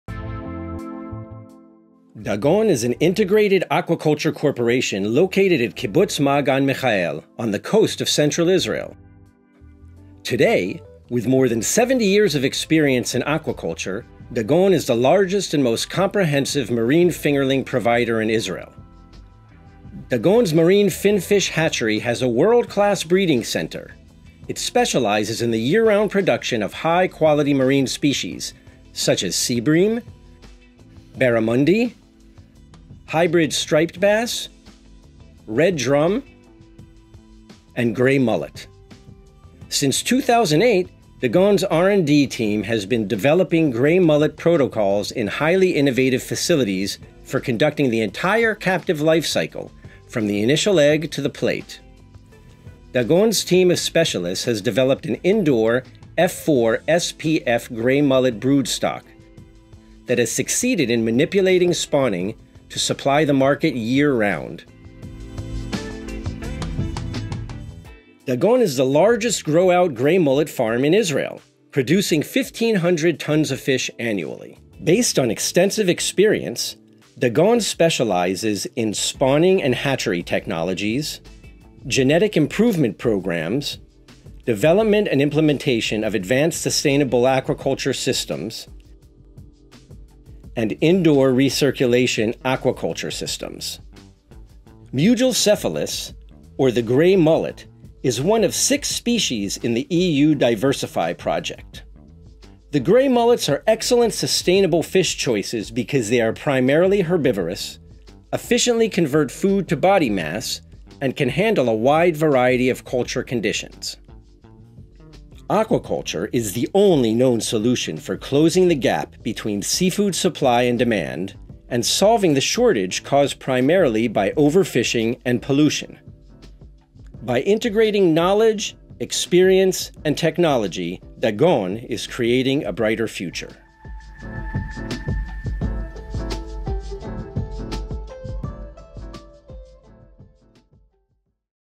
Dagon Aquaculture
From humble beginnings in the 1980s at Kibbutz Ma’agan Michael, Dagon has grown into a global leader in the ornamental fish industry. Starting with Koi and Goldfish, our commitment to quality and bio-secure practices has made us a top supplier worldwide. 🐟✨

Over the past 20 years, we’ve expanded our expertise, venturing into new markets and developing cutting-edge breeding lines. Nestled along the shores of the Mediterranean Sea, our Marine Fish Hatchery now nurtures and raises a diverse range of marine finfish, continuing our legacy of excellence in aquaculture. 🌍🌱